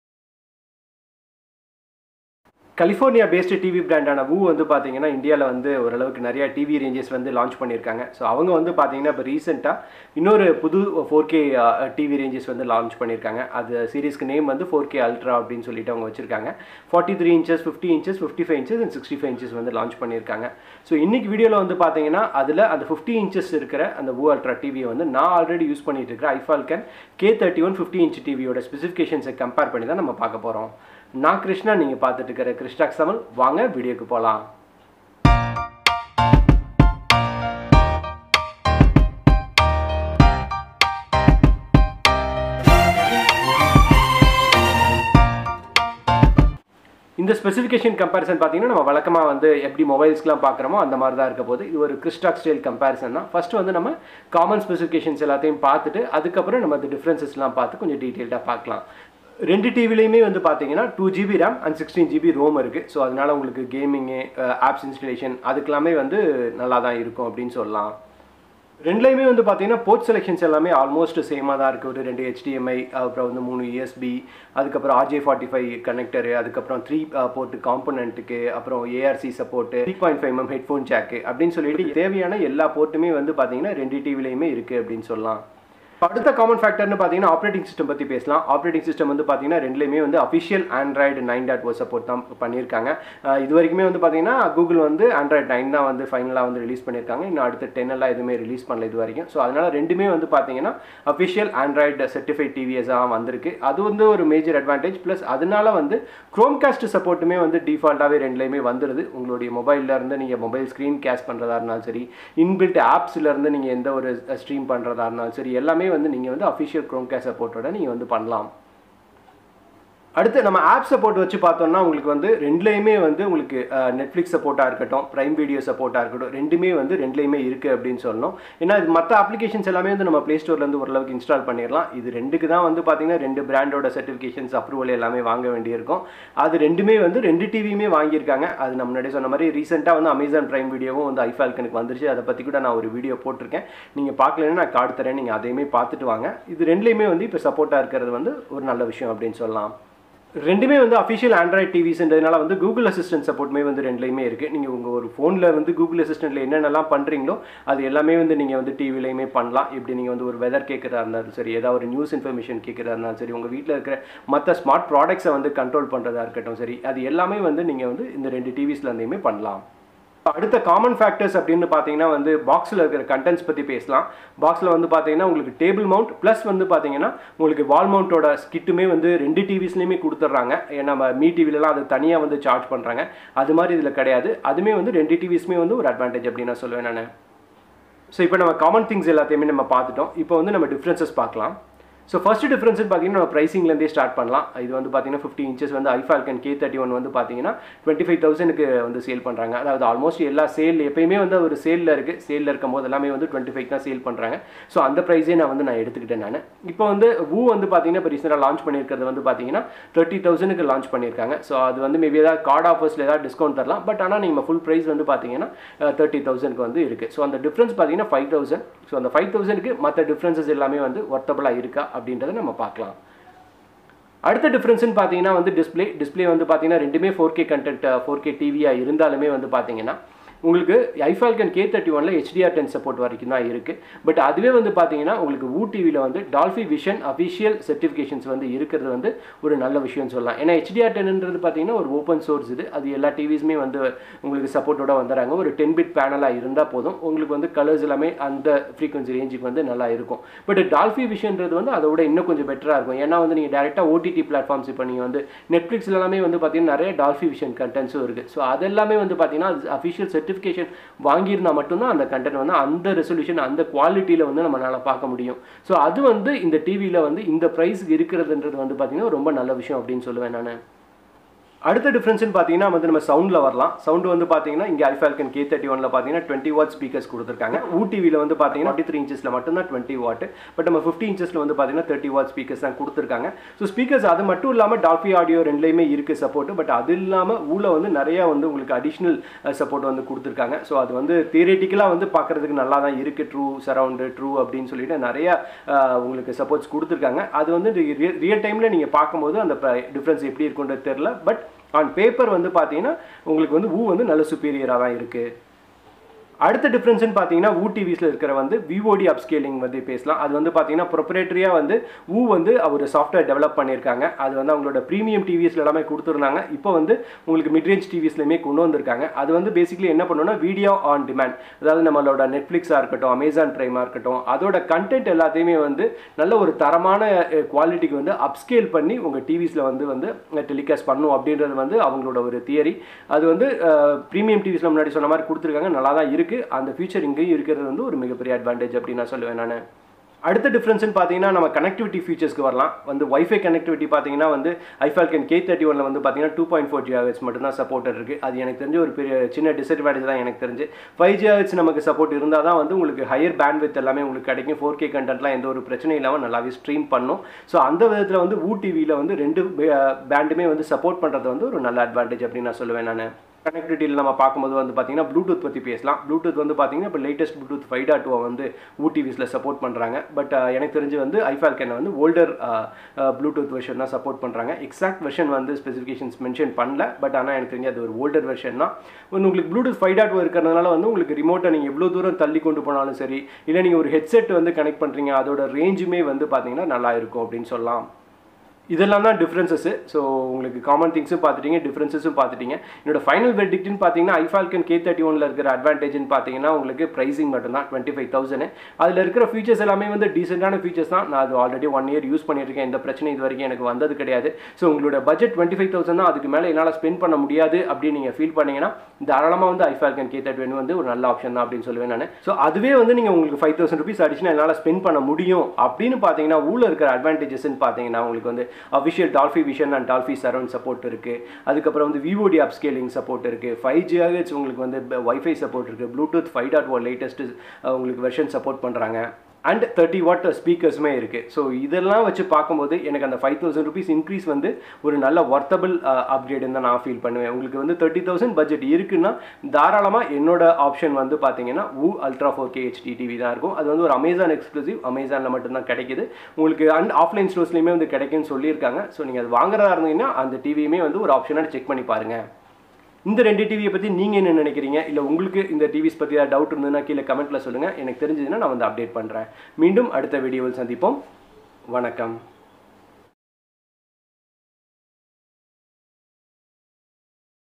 Vu Ultra vs iFFALCON K31 - Specifications Comparison [ Tamil ]
Vu has launched new series of 4K TVs in 43, 50, 55 & 65 inches.

They have named series as Ultra 4K TVs.

I am already using iFFALCON K31 50inch TV in my home.

In this video i have compared specifications between the two and i have shared my thoughts.

These Images used in the video are compliance with fair dealing usage permitted under Sec. These are not used for any commerical purpose.